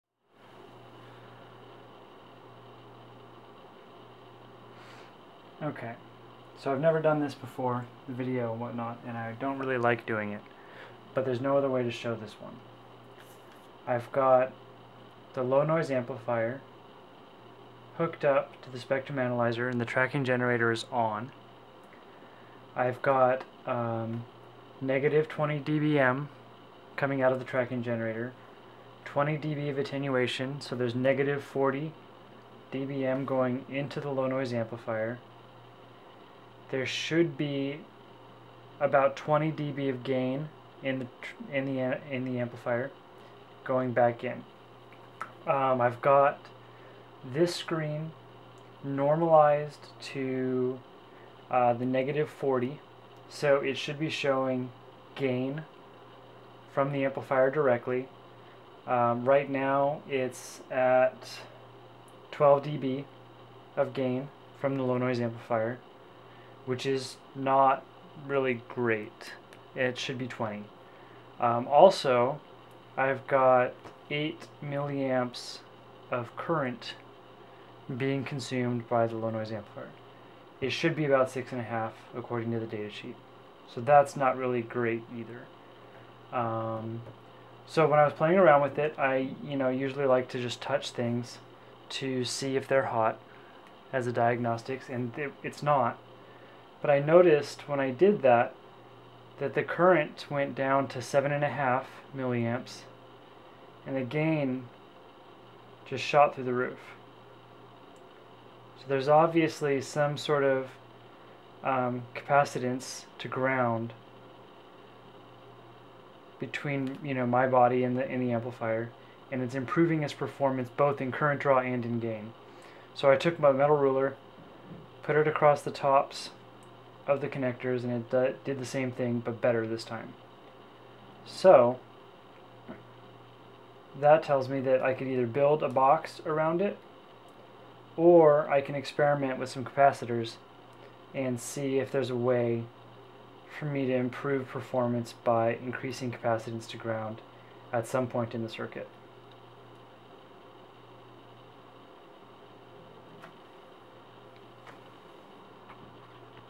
LNA testing
Testing my Skyworks 65047-based low noise amplifier.